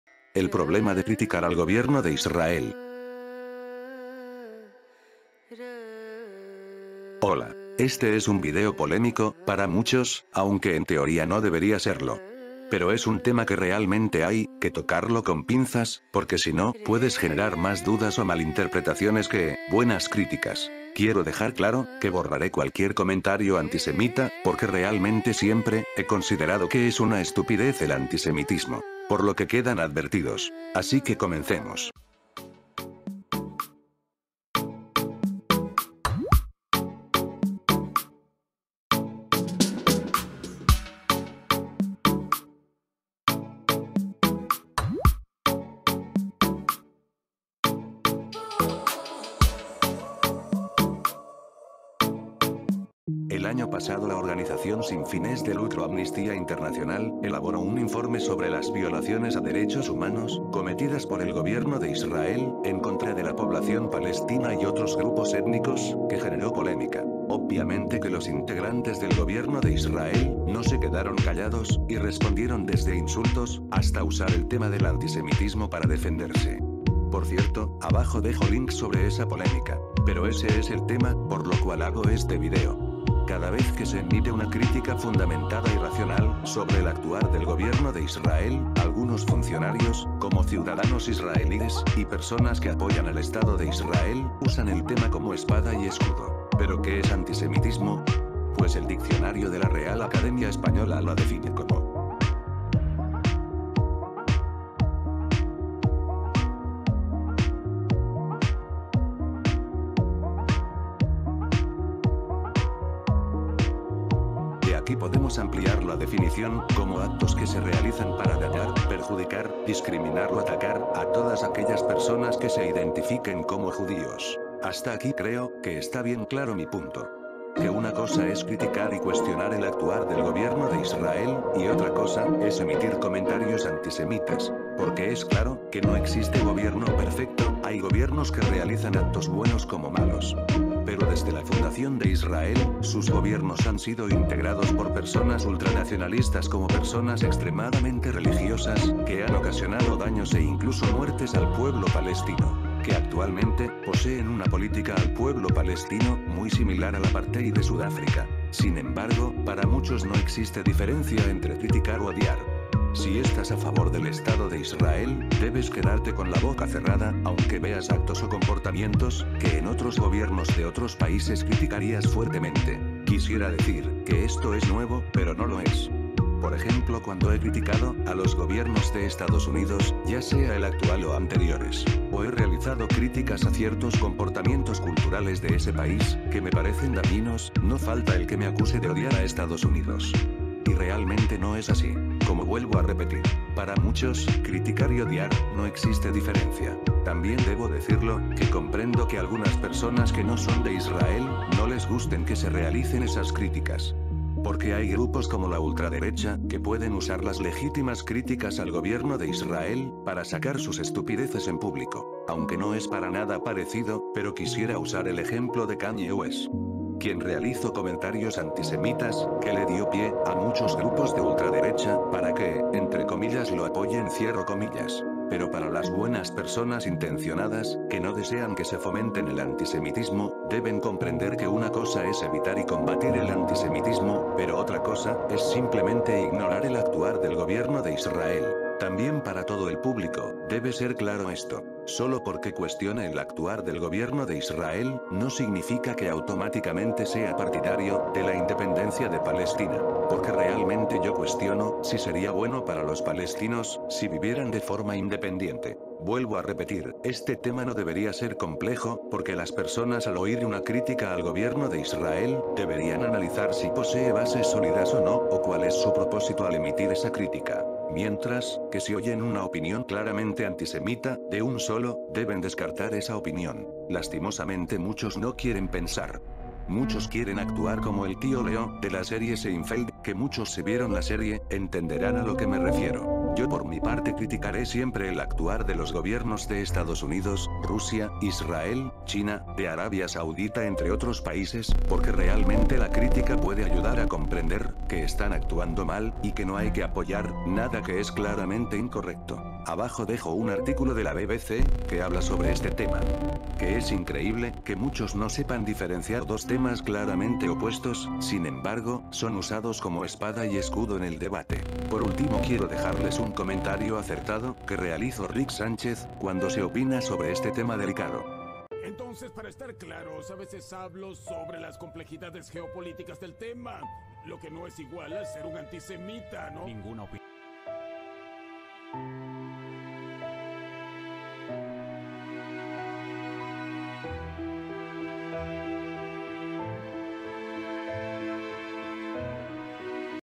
El problema de criticar al gobierno de Israel.
Solo para algunos es un tema muy polémico.